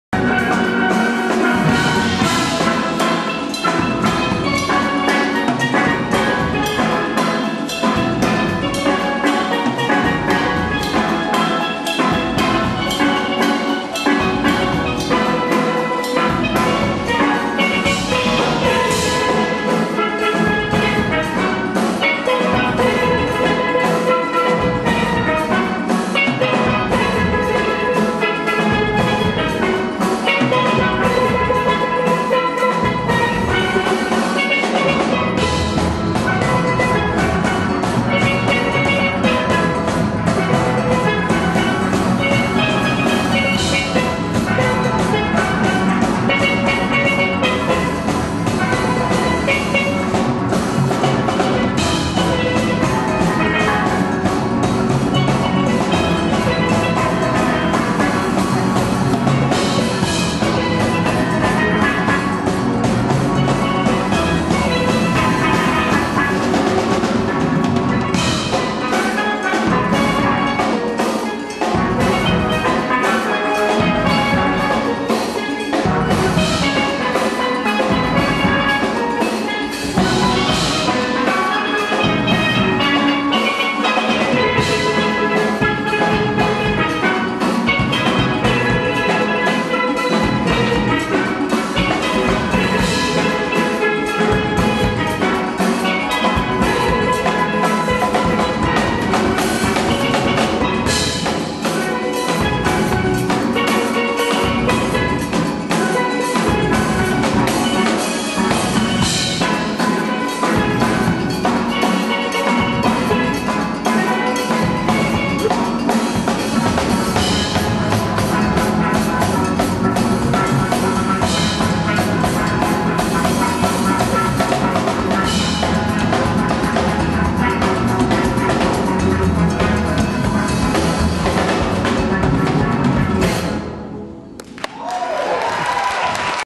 Miami Pan Symphony 2007 at Florida Memorial College
Miami Pan Symphont performs at a benefit concert at the Florida Memorial College 2007